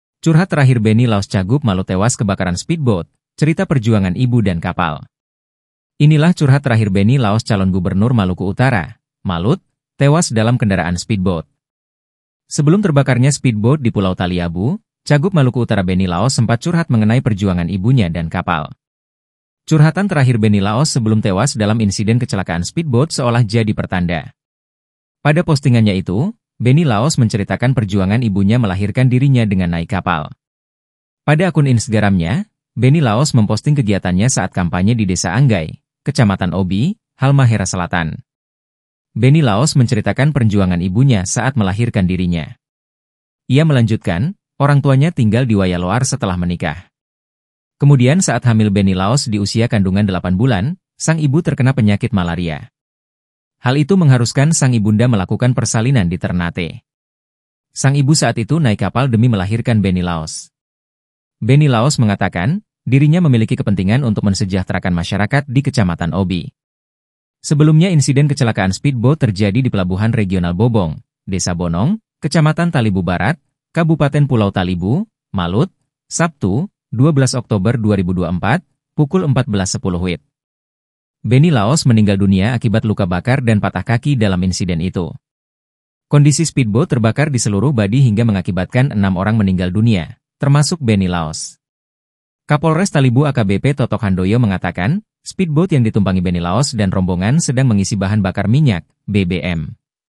CURHAT Terakhir Benny Laos Cagub Malut Tewas Kebakaran Speedboat, Cerita Perjuangan Ibu dan Kapal
Inilah curhat terakhir Benny Laos calon Gubernur Maluku Utara (Malut) tewas dalam kendaraan speedboat.

Sebelum terbakarnya speedboat di Pulau Taliabu, Cagub Maluku Utara Benny Laos sempat curhat mengenai perjuangan ibunya dan kapal.

Lihat informasi terkini bersama kami, di:

Palembang.tribunnews.com

Sumber Video : Tribunnews.com, benny.laos, x/ @panca66
Editor Video : Sarah